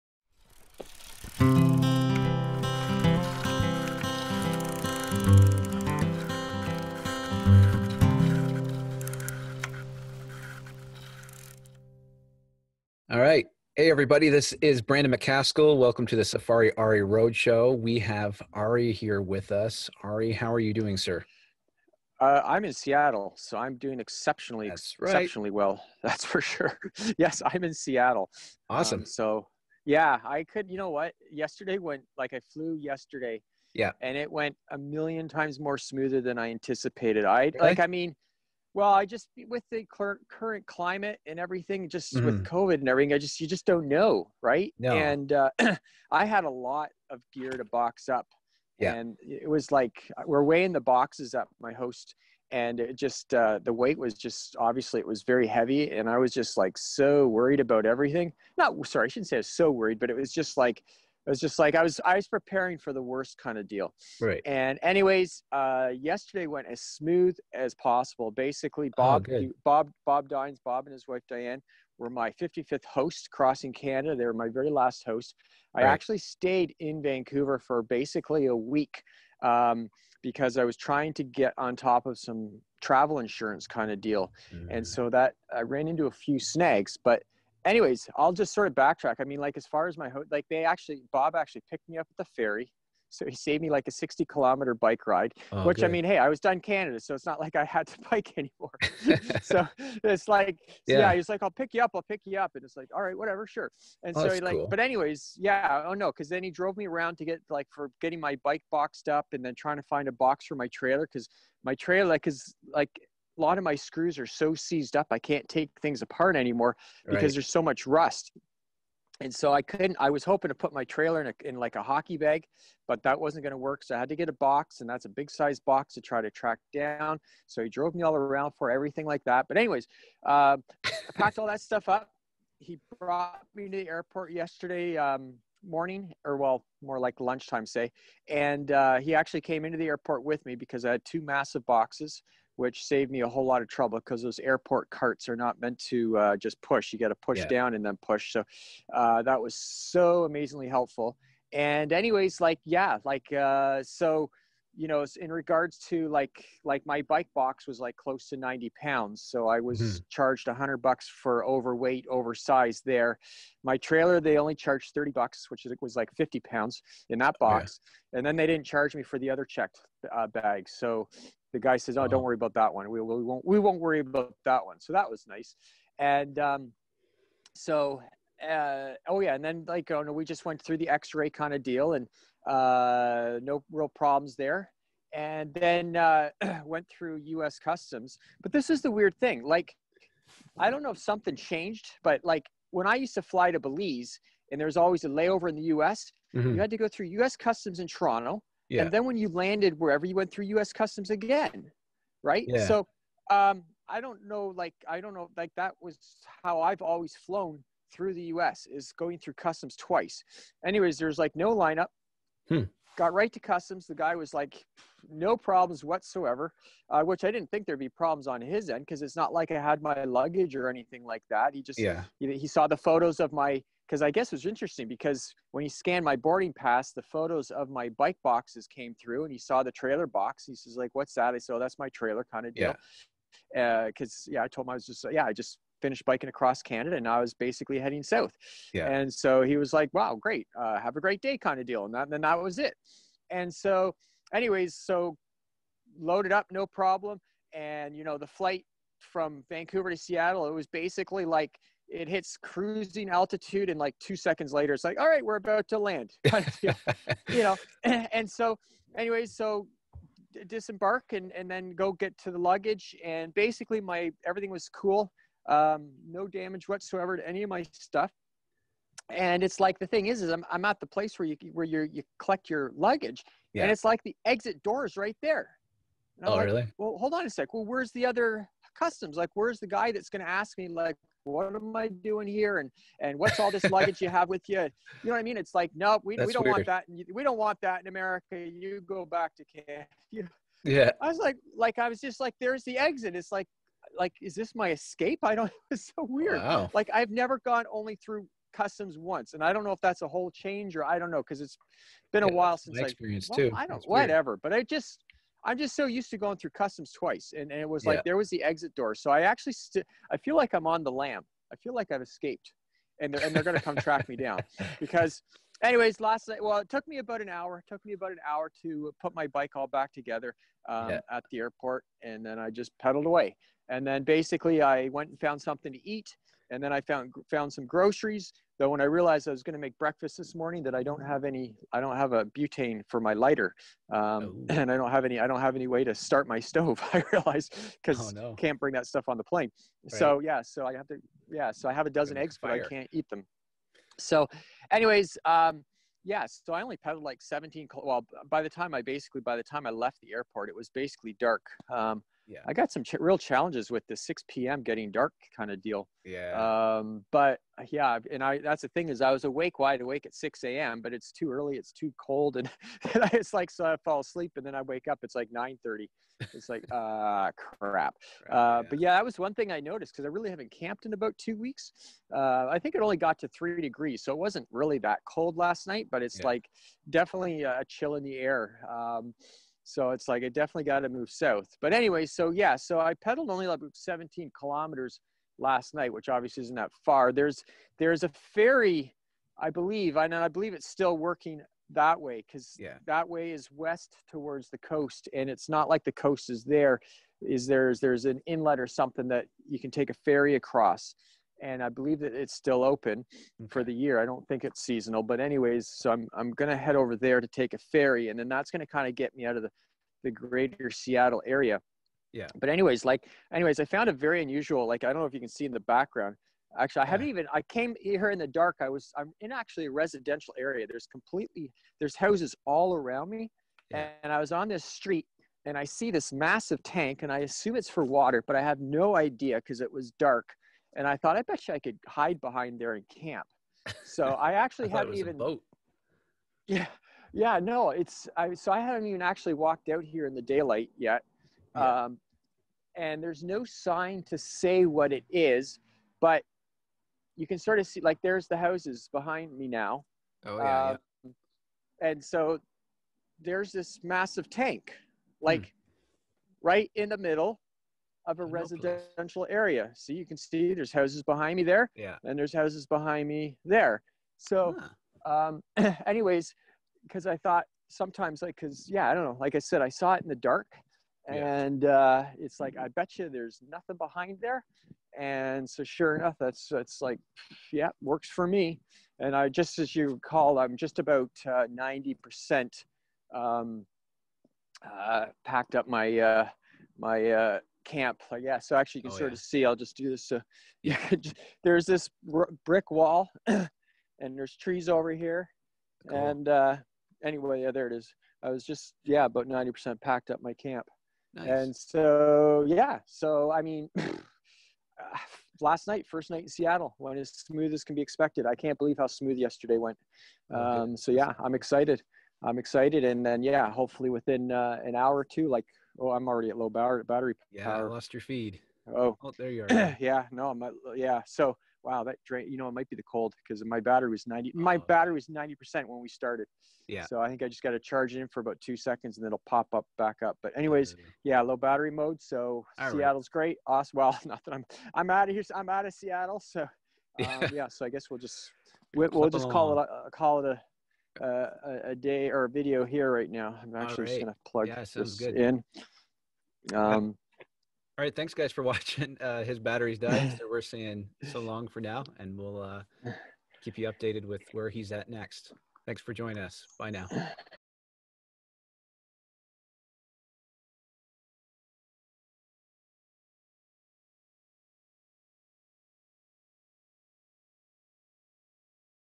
I arrived in Seattle!! The Safari Arie Road Show
My arrival into the US.  The blog post that lines up with this video can be found

Safari Arie
I started November 12, 2019.  To bike 50,000 km’s across Canada and through every country in all 3 Americas to the bottom of Argentina.